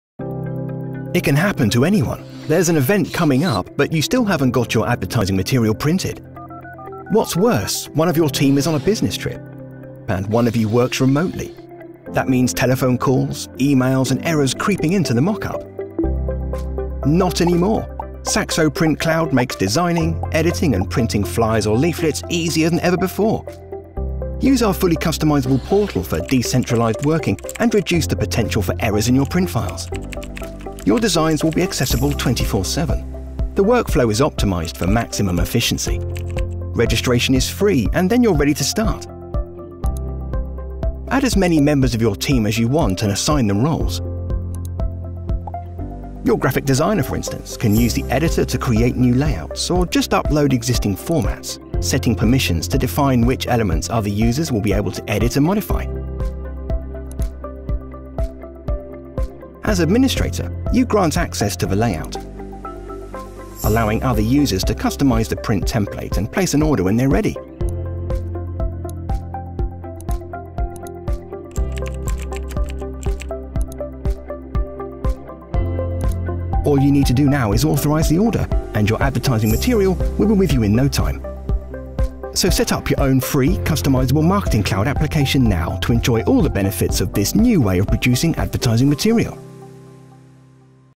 SAXOPRINT.cloud - Your free teamwork and marketing application
– The first free, team-based marketing portal: for efficient marketing and a cost-optimised workflow

Are you a decentralised business that spends a lot of time coordinating print jobs? Are mistakes constantly creeping in that you want to avoid in the future? Then the SAXOPRINT.cloud is just the thing. The free marketing platform involves the whole team and simplifies processes from the creation of layouts to the printing of your advertising materials.

SAXOPRINT.cloud offers you:

- a personalised portal for decentralised working
- fewer sources of error when creating print data
- 24 hour access to advertising media
- efficiency - an optimised workflow

Simply register for free and try it out. In just a few minutes, you will be able to invite your team members and assign roles to them. Thanks to the assignment of rights in the integrated graphic Editor, the corporate design of your prints is always retained.

The SAXOPRINT.cloud is always there to help you wherever advertising material needs to be printed.

Work quickly, efficiently and with full cost transparency: from layout to finished product.